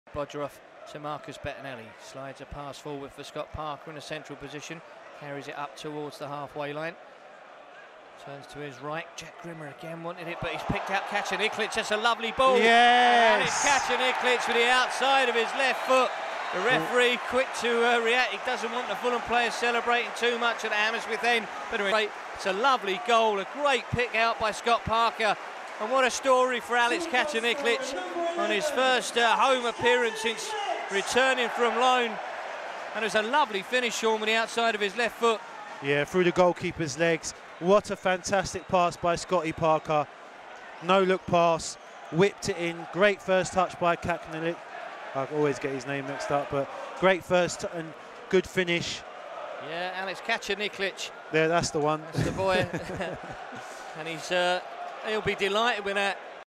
44. Alexander Kačaniklić, Reading (H)
Alexander Kačaniklić scored his first goal since his return from a loan spell at FC Copenhagen to put the Whites on their way against the Royals.